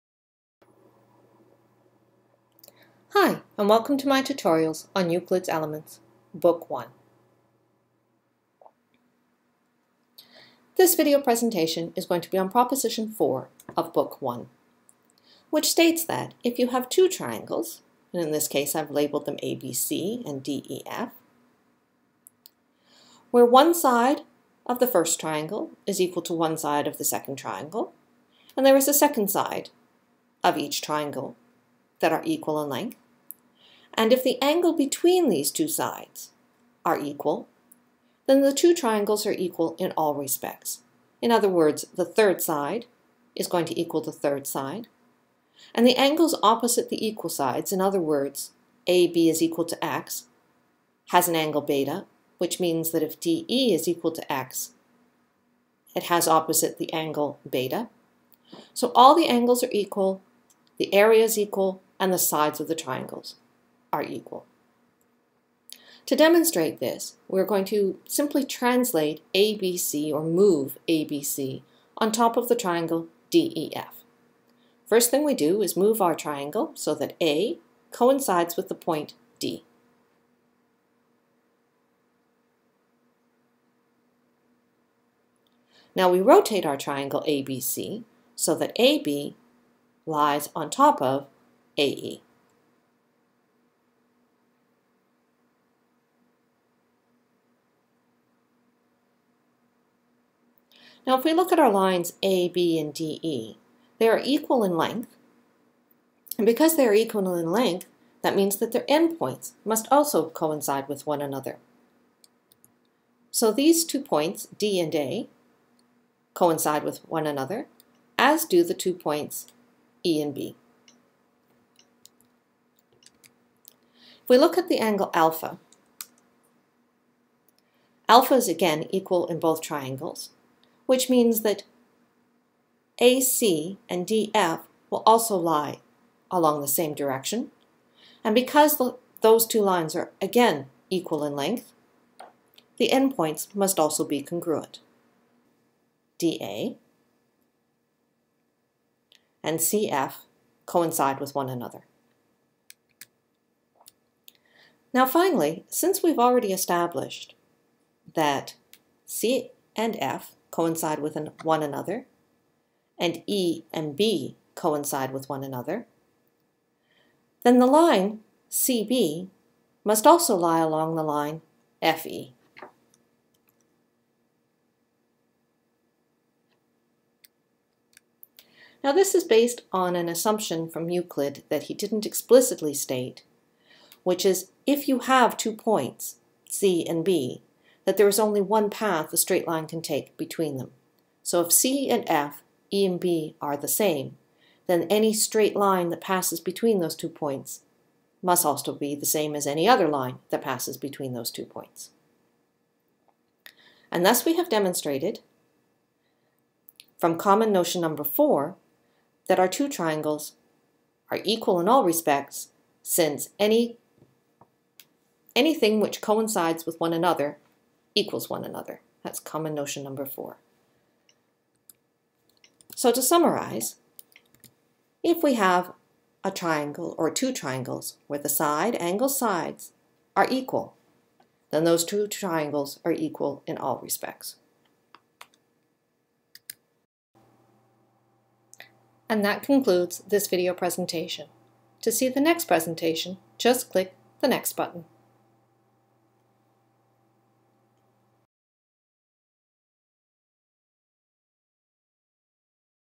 Euclid's Elements Book 1 - Proposition 4
If a triangle has two sides equal to two sides in another triangle, and the angle between them is also equal, then the two triangles are equal in all respects